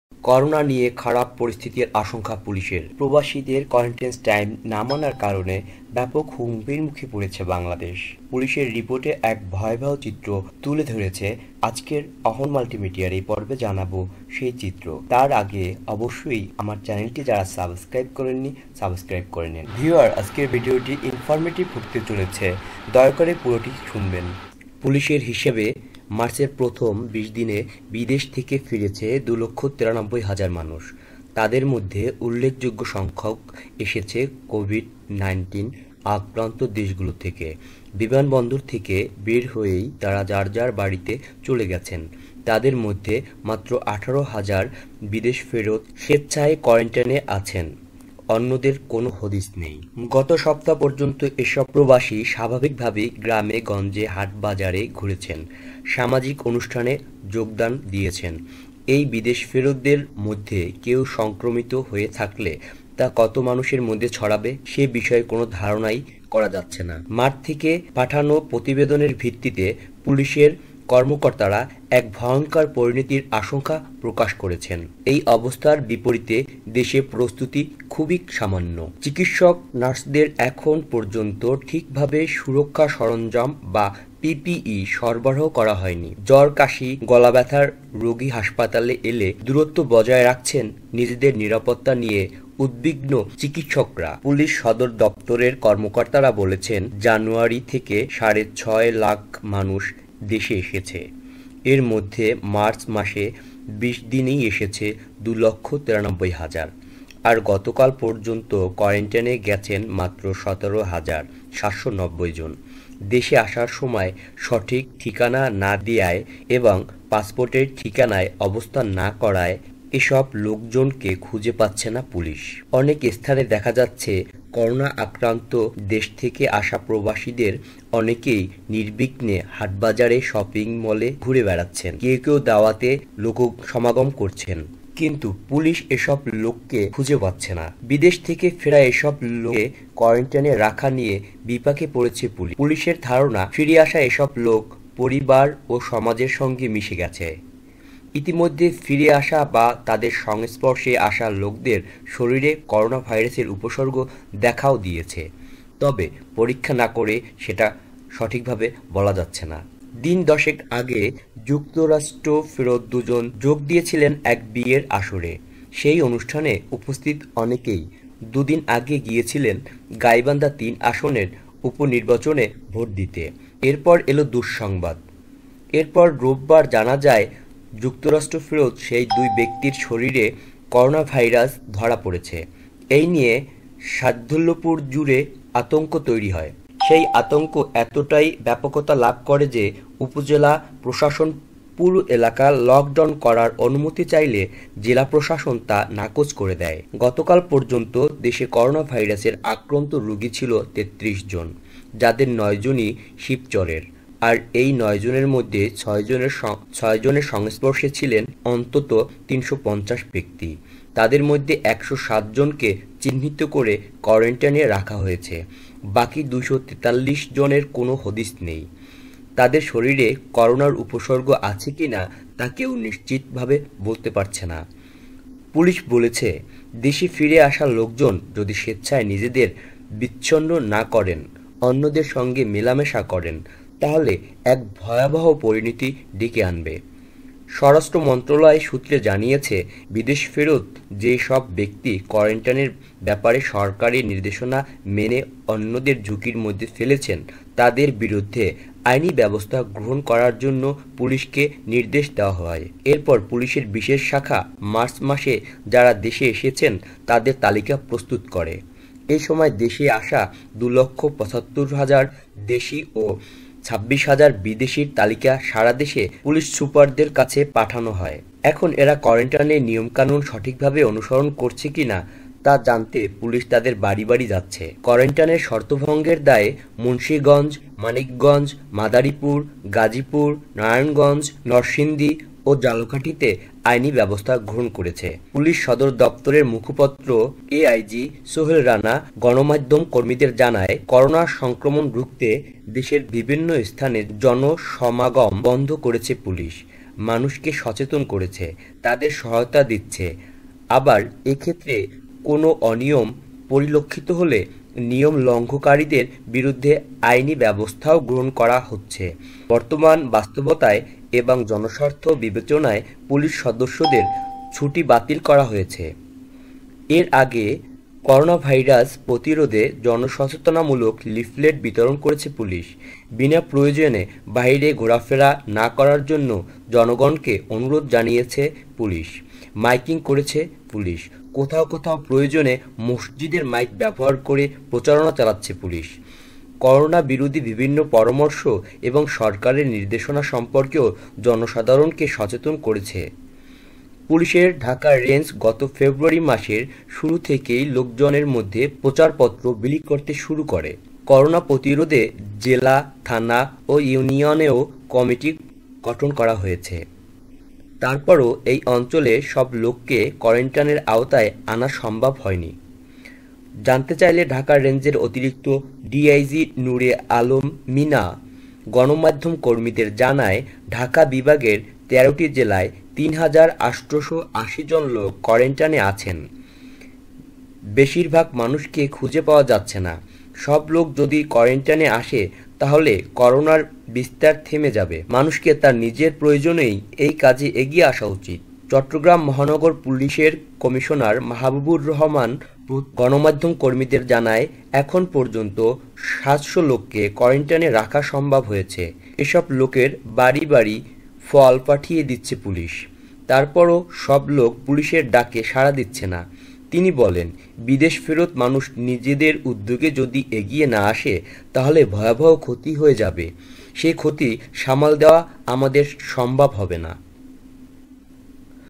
করোনা নিয়ে ‘খারাপ পরিস্থিতির’ আশঙ্কা পুলিশের || বাংলাদেশ আছে উচ্চ ঝুঁকিতে
পুলিশের হিসাবে মার্চের প্রথম ২০ দিনে বিদেশ থেকে ফিরেছেন ২ লাখ ৯৩ হাজার মানুষ। তাঁদের মধ্যে উল্লেখযোগ্য সংখ্যক এসেছেন কোভিড-১৯ আক্রান্ত দেশগুলো থেকে। বিমানবন্দর থেকে বের হয়েই তাঁরা যাঁর যাঁর বাড়িতে চলে গেছেন। তাঁদের মধ্যে মাত্র ১৮ হাজার বিদেশফেরত স্বেচ্ছা কোয়ারেন্টিনে আছেন। অন্যদের কোনো হদিস নেই।

দৈনিক প্রথম আলোতে
স্ক্রিপ্ট সংগৃহীত দৈনিক প্রথম আলো পত্রিকা থেকে